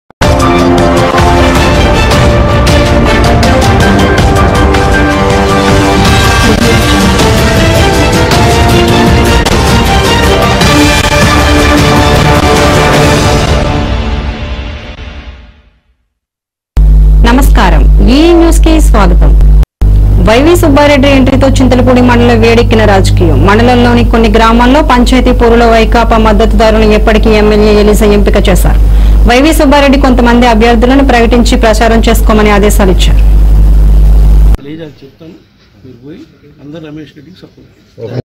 వైవీ.సుబ్బారెడ్డిఎంట్రీతో చింతలపూడి లో వేడెక్కిన రాజకీయం మండలంలోని కొన్ని గ్రామాల్లో పంచాయతీ పోరులో
వైవీ.సుబ్బారెడ్డి ఎంట్రీతో చింతలపూడి మండలంలో వేడెక్కిన రాజకీయం మండలంలోని కొన్ని గ్రామాల్లో పంచాయతీ పోరులో